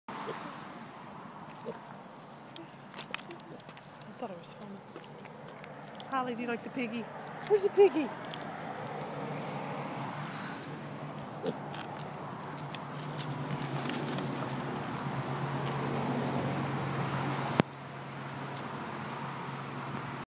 Holly Meets Kielbasa
Our dog Holly meeting our neighbor's pot-bellied pig Kielbasa.